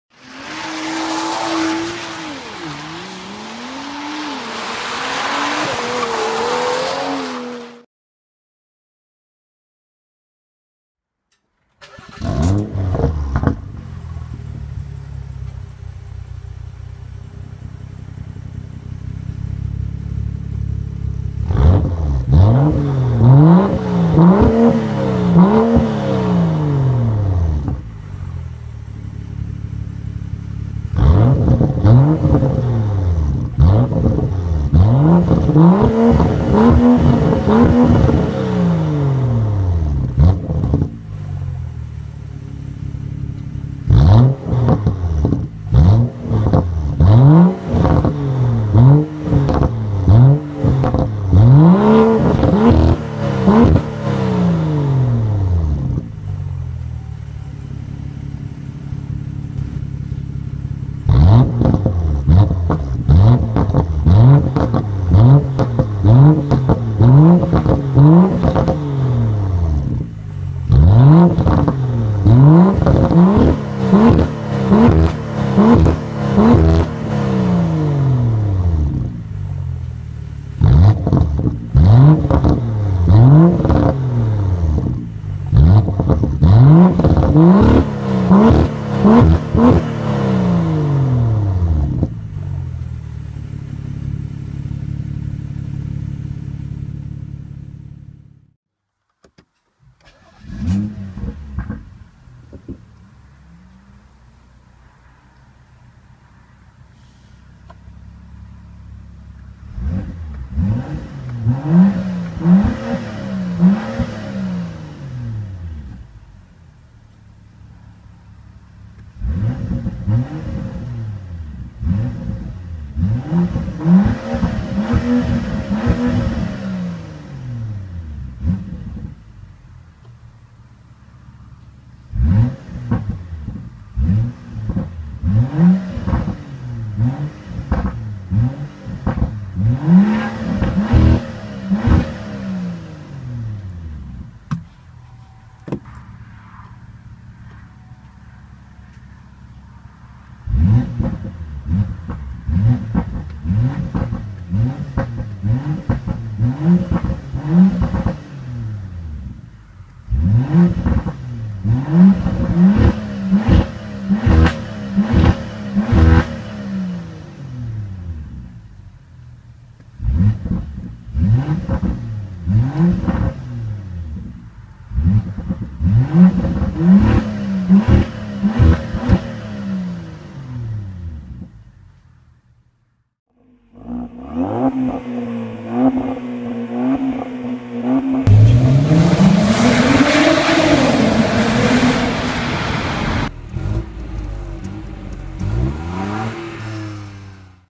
2017 X4 M40i Stock Exhaust Sound All Modes compared | BMW F26
Exhaust sound of:
2017 BMW F26 X4 M40i
Completely stock
N55B30 (3.0 I6 Turbo)
360 HP / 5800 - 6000 rpm (265 kw)
465 Nm / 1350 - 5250 rpm
ZF 8 speed Sport Automatic
xDrive (All Wheel Drive)
1920 kg
Summer tires
0 - 100 km/h: 5.3 sec (wet road)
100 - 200 km/h: 14.9 sec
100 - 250 km/h: 40.1 sec

Practical for car wash, cleans all gaps:

I use this tripod for my videos:
- it is fixed quickly
- magnetic feet
- fits into the trouser pocket